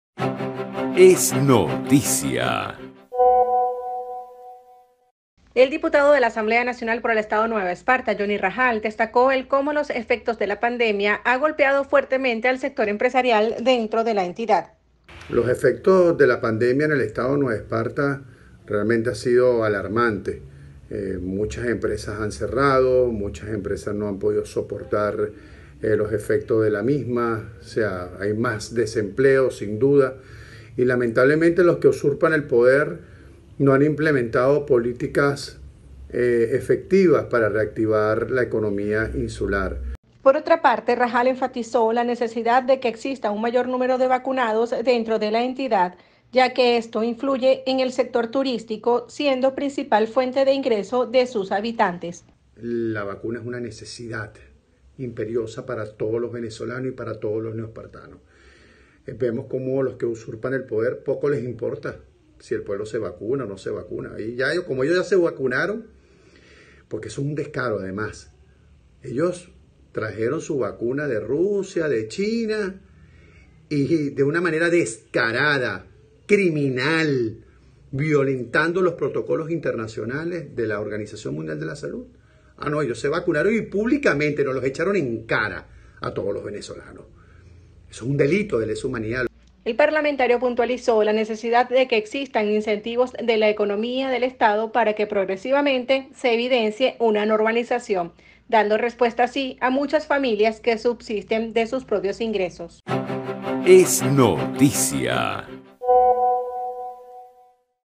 Dip. Jony Rahal destacó como la pandemia afectó al sector empresarial en Nueva Esparta
El Dip. Jony Rahal destacó como la pandemia ha golpeado fuertemente al sector empresarial en Nueva Esparta.

“Muchas empresas han cerrado, no han podido soportar los efectos de la misma, hay más desempleo y los que usurpan el poder no implementan políticas efectivas”.